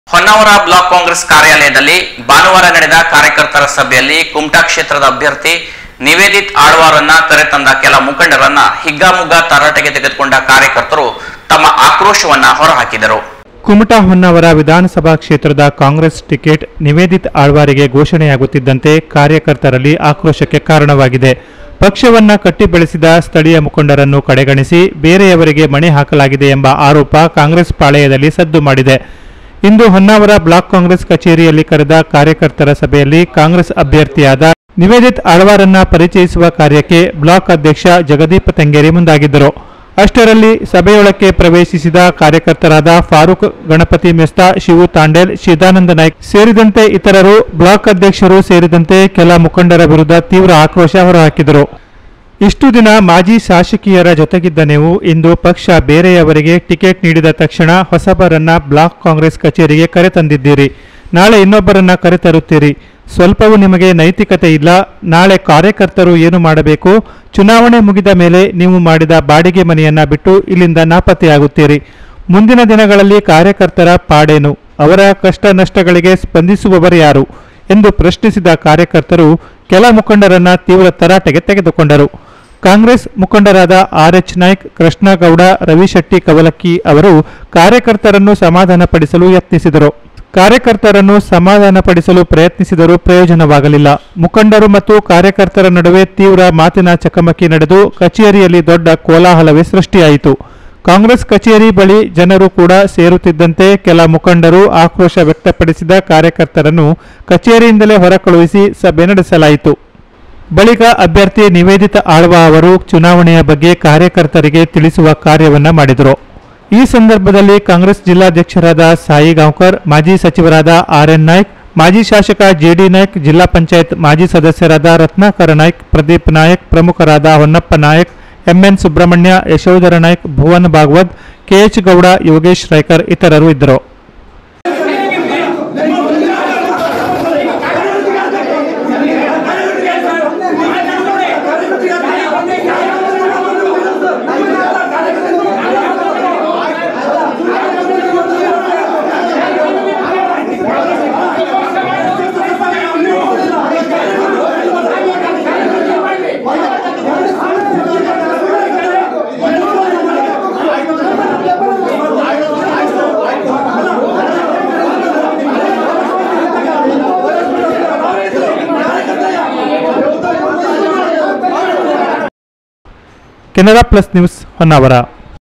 ಹೊನ್ನಾವರ ಬ್ಲಾಕ್ ಕಾಂಗ್ರೆಸ್ ಕಾರ್ಯಾಲಯದಲ್ಲಿ ಭಾನುವಾರ ಹಿಗ್ಗಾಮುಗ್ಗಾ ತರಾಟೆಗೆ ತೆಗೆದುಕೊಂಡ ಕಾರ್ಯಕರ್ತರು
16-4-23 ಹೊನ್ನಾವರ ಬ್ಲಾಕ್ ಕಾಂಗ್ರೆಸ್ ಕಾರ್ಯಾಲಯದಲ್ಲಿ ಭಾನುವಾರ ನಡೆದ ಕಾರ್ಯಕರ್ತರ ಸಭೆಯಲ್ಲಿ ಕುಮಟಾ ಕ್ಷೇತ್ರದ ಅಭ್ಯರ್ಥಿ ನಿವೇದಿತಾ ಆಳ್ವಾ ಅವರನ್ನು ಕರೆತಂದ ಕೆಲ ಮುಖಂಡರನ್ನು ಹಿಗ್ಗಾಮುಗ್ಗಾ ತರಾಟೆಗೆ ತೆಗೆದುಕೊಂಡ ಕಾರ್ಯಕರ್ತರು ತಮ್ಮ ಅಸಮಧಾನವನ್ನು ಹೊರಹಾಕಿದರು. canaranews uttarakannad uknews uttarakannadasuddi uttarakannadanews breakingnews latestnews importantnews suddi livenews tvlive livechannel aja ka news